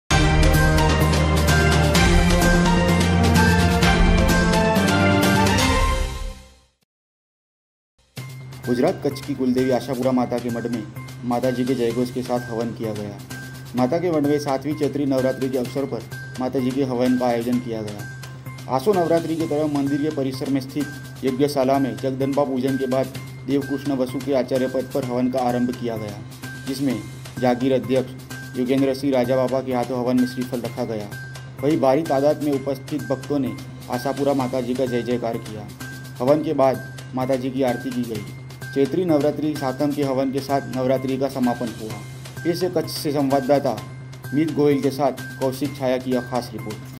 CNCC TV NEWS--गुजरात कच्छ की कुलदेवी आशापुरा माता के मढ में हवन का आयोजन हुवा
गुजरात कच्छ की कुलदेवी आशापुरा माता के मढ में हवन का आयोजन हुवा.